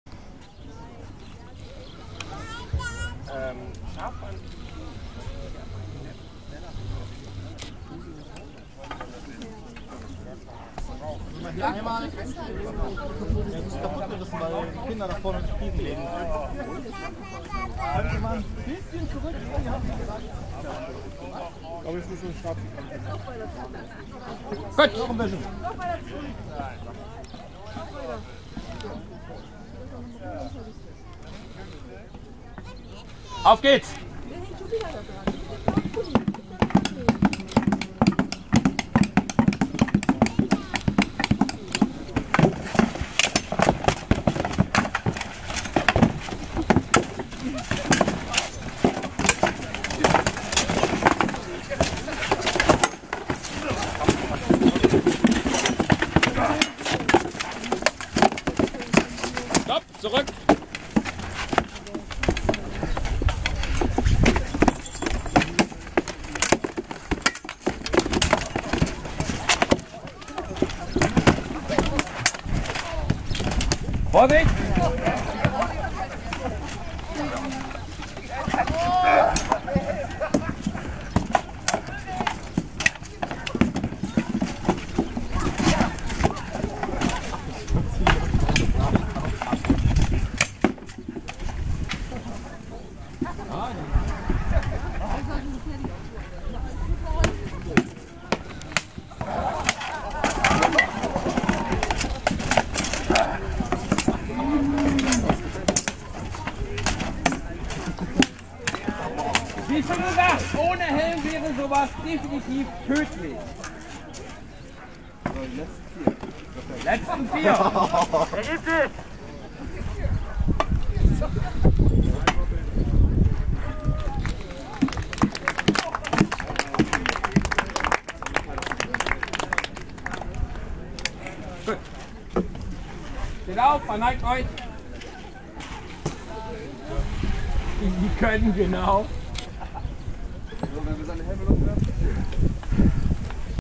Tamme Hanken's Sommerfest Melee Runde 1
Einige Leute aus unserer Reenactmentgruppe; Volk des heiligen Reiches beim Sommerfest von Tamme Hanken. Und ja wir haben einige Beulen abbekommen :P